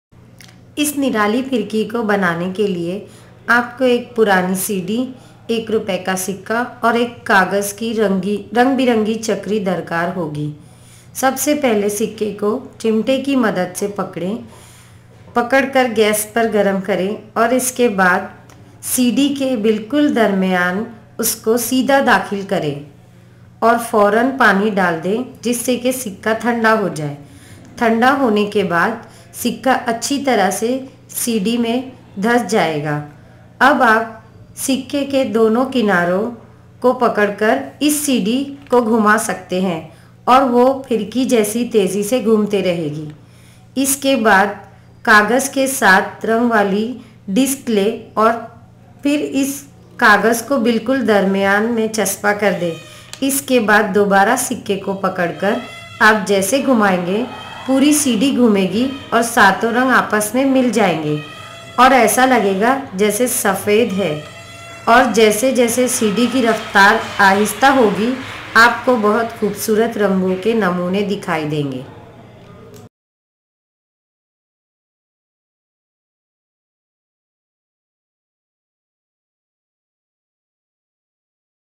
CD COIN SPINNER | Urdu
To make this amazing spinner you require a coin, an old CD and a seven color Newton’s Disc. Hold a coin with pliers and heat it on a gas flame. TAKE ADULT HELP.
Then insert the hot coin vertically in the centre of an old CD. Immediately pour water to cool down. This way the coin will be firmly fixed to the CD without any glue. Now grip the coin and give it a twirl. This will make the CD spin like a top for a very long time. It is good top with a low centre of gravity and a large moment of inertia. Now apply glue on the backside of the Newton’s Color Disk and stick it on top of the CD.
Once again grip the coin and spin the CD. The different colors will mix and you will see a grayish-white. As the CD slows down you will see many beautiful color patterns.

Dubbed in Urdu by Naziya Shaik from Pratham's Science Learning Program